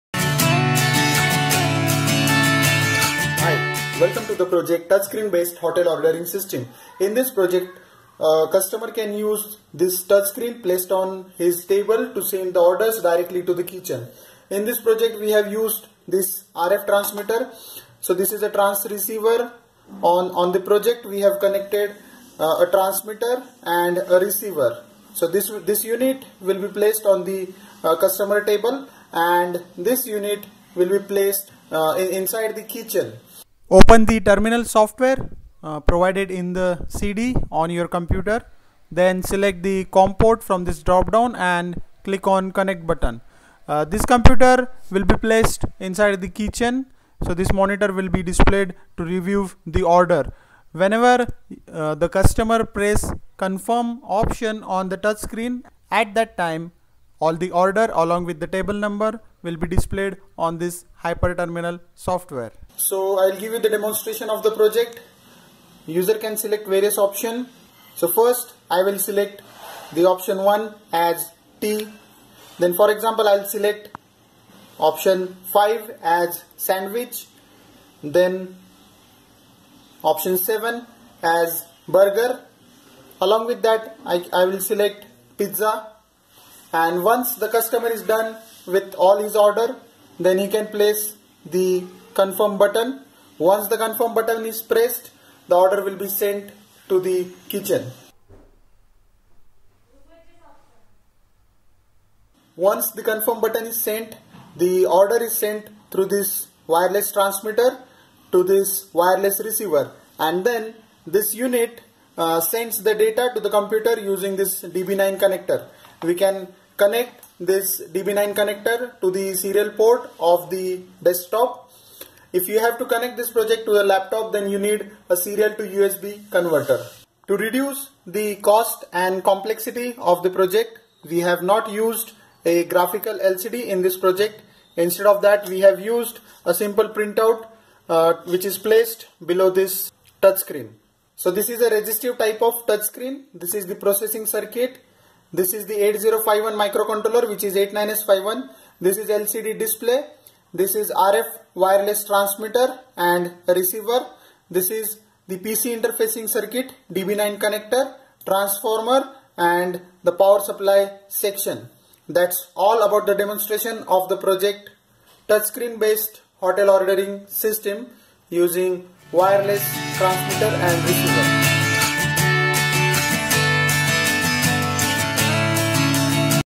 Touchscreen based wireless hotel ordering system
Touchscreen is placed on the table of customer in hotel. Customer can place the order using the touch screen panel. Order will be sent directly to kitchen using wireless transmitter. Hyper terminal software should be used on computer to view the order. Please note that we have not used any graphical display in this project to reduce the cost and complexity of the project.